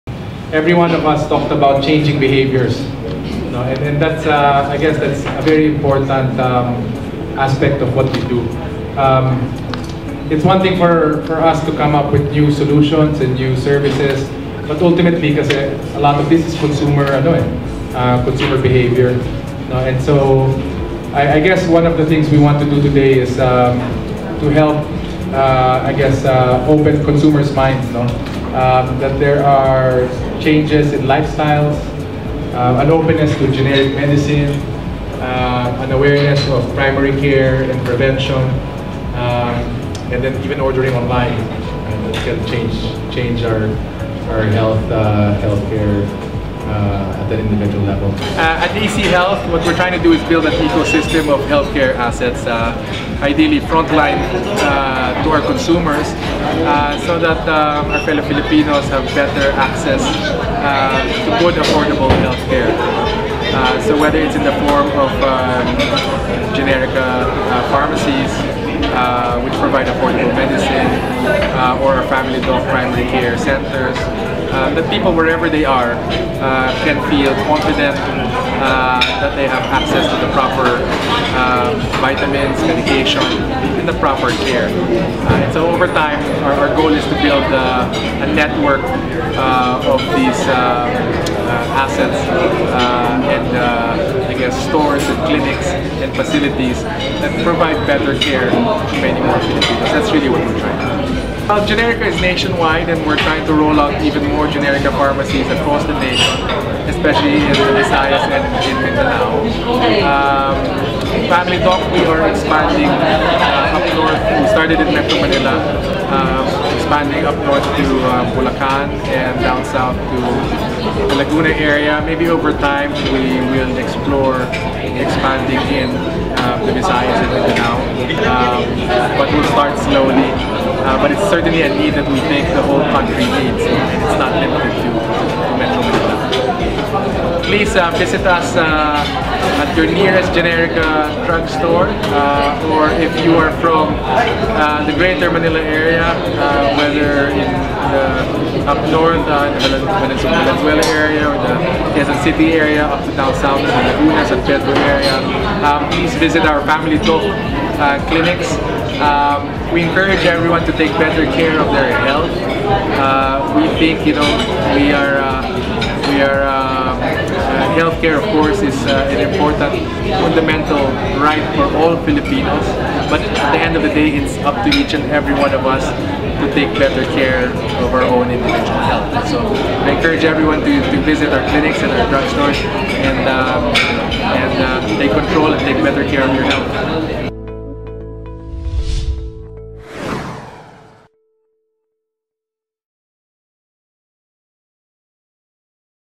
How Ayala Healthcare helps in delivering healthcare service to Filipino people
Ayala Healthcare Holdings embark on a “blue ocean strategy” in the healthcare service by coming up with the first community-based primary care clinics, offering the combined services of a clinic,a diagnostic facility and a pharmacy all under one roof.